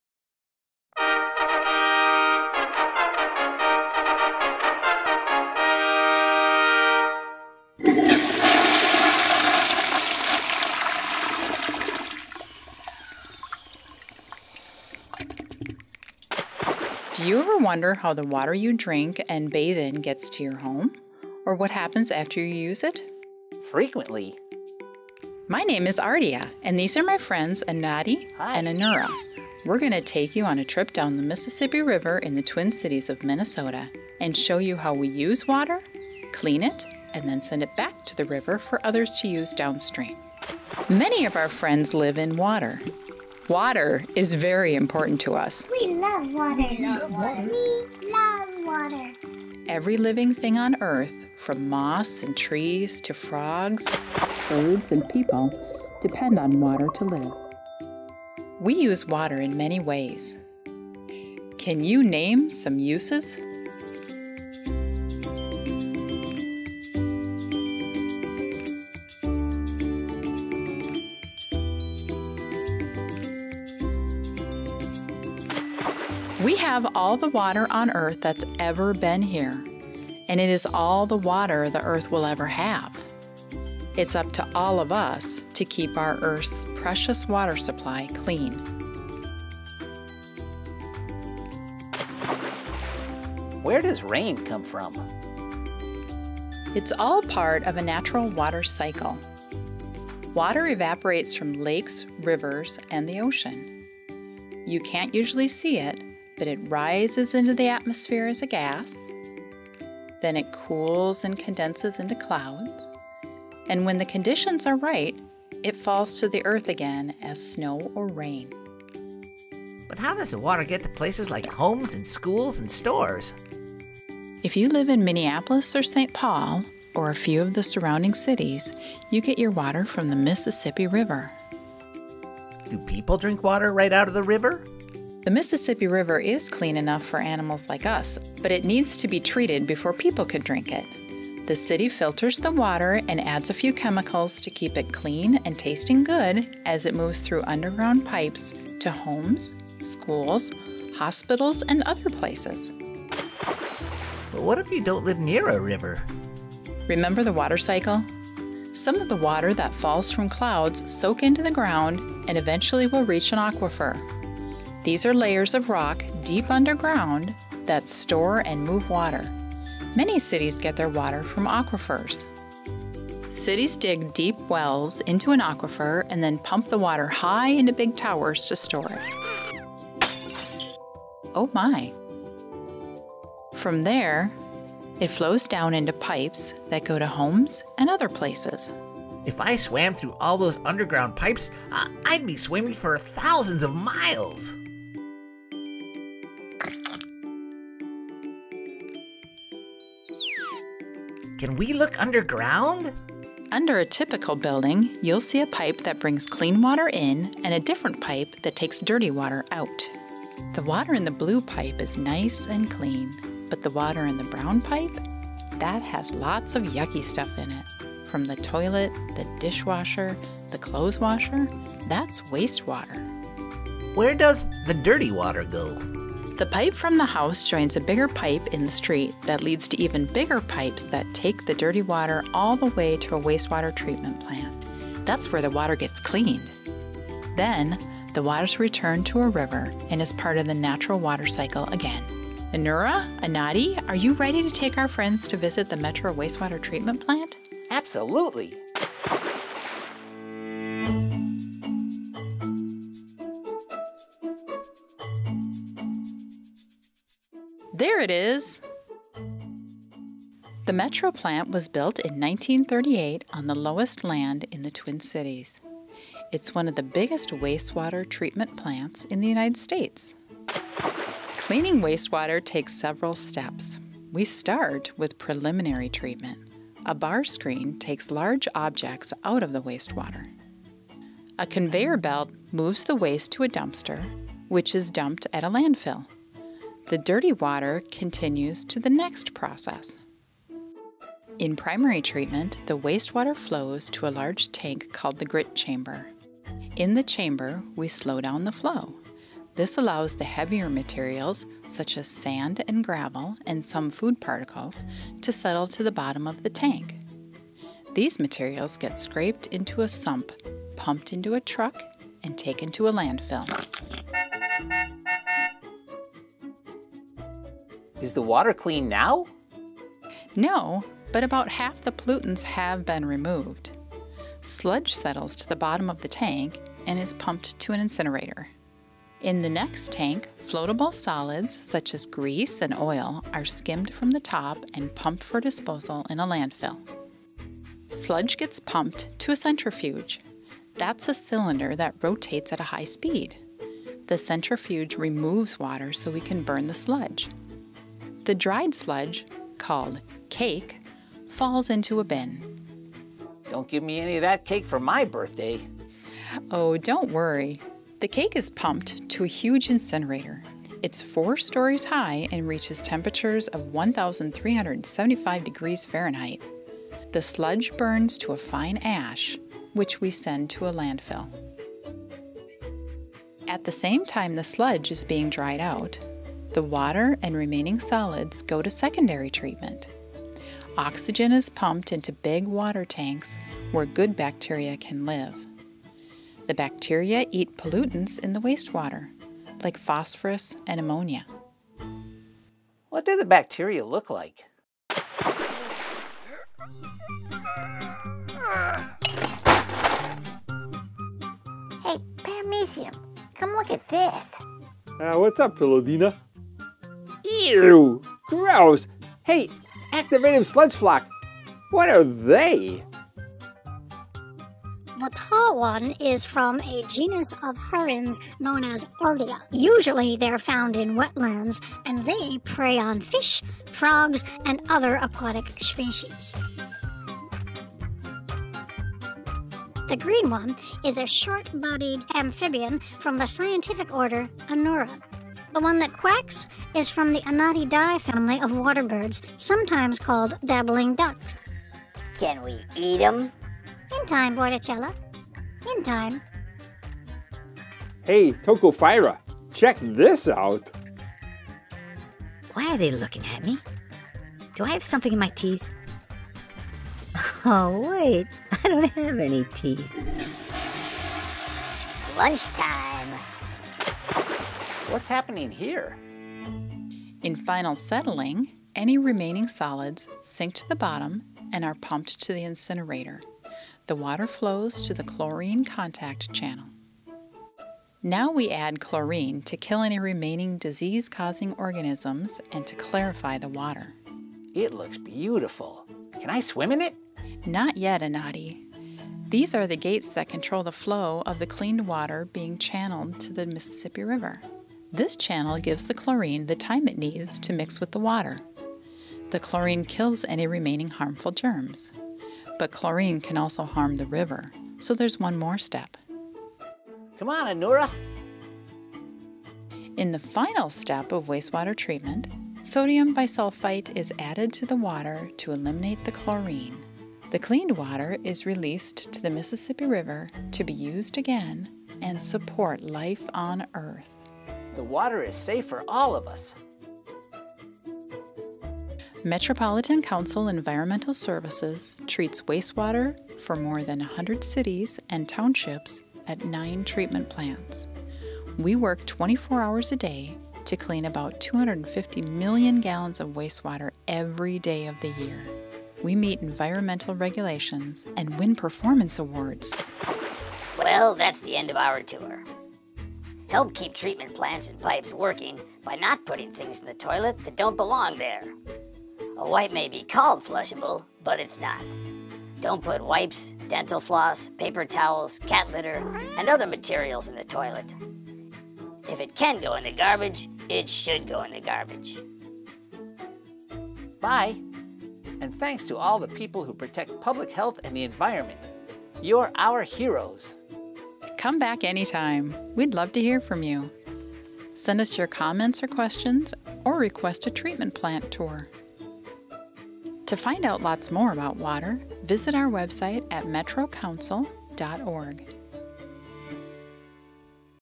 The Metropolitan Council presents Wastewater Treatment for Kids
The water that people use in their homes and businesses comes from nature. An important part of the water cycle is cleaning and returning it to nature so it can be used again.

Animals and plants that live in our rivers and lakes need clean water to live in, to drink, and to grow the foods they eat. Cleaning wastewater (sewage) protects the environment and protects public health.

Learn about the water cycle and take a look behind the scenes of how Metropolitan Council's Environmental Services cleans and returns water to nature.

0:00  Open
0:40  We Love Water
1:32  Where does rain come from?
2:00  How does water get to homes and buildings?
2:33  What if you don’t live near a river?
3:22  Look underground!
4:15  Visit the Wastewater Treatment plant!
4:31  Preliminary treatment
4:51  Primary Treatment
5:38  Making cake!
6:12  Secondary treatment
6:35  Bacteria!
7:43  Final settling
7:57. Chlorine contact channel
8:29  Last step to cleaning the water
8:48   Metropolitan Council Environmental Services
9:10  How you can help protect our water!
9:53  Send comments or questions or even request a treatment plant tour!